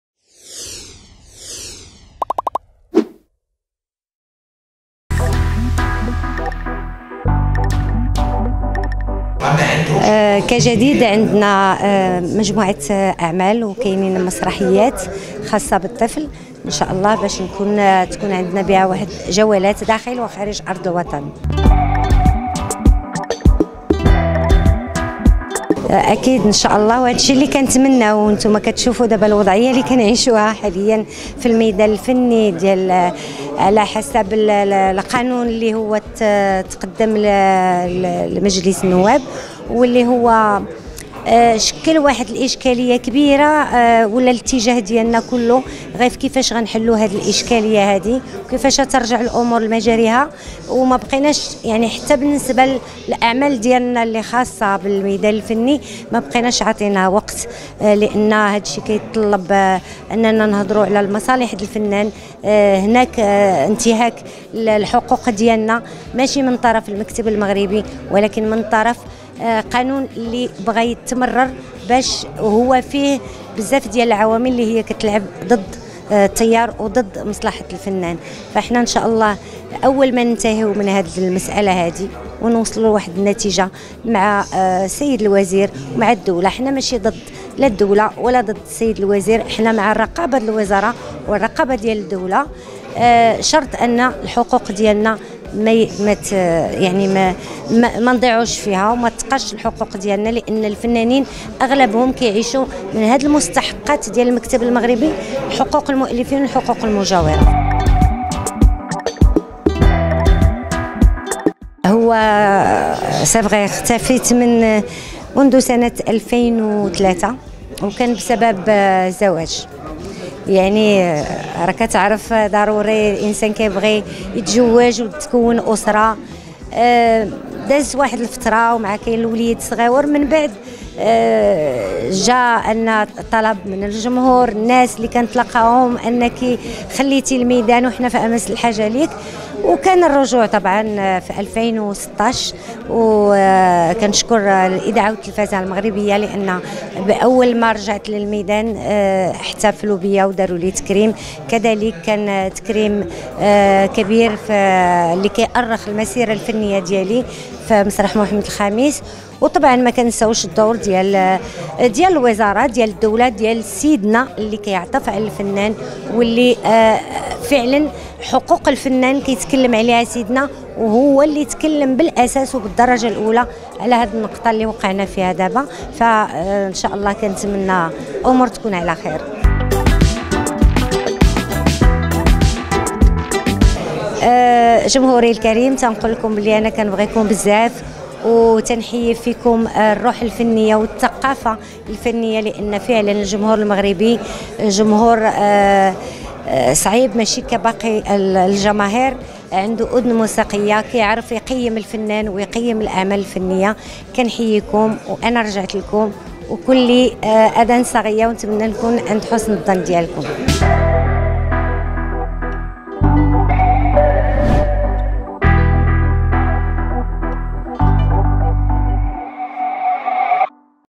بعد غياب دام 13 سنة.. صباح الشنة تفتح قلبها لمجلة فنانة وتكشف جديدها وسبب غيابها
مجلة فنانة تابعة للمجموعة الإعلامية "ميديا كونفدونس" تهتم بإبداعات الشباب والنساء في مختلف المجالات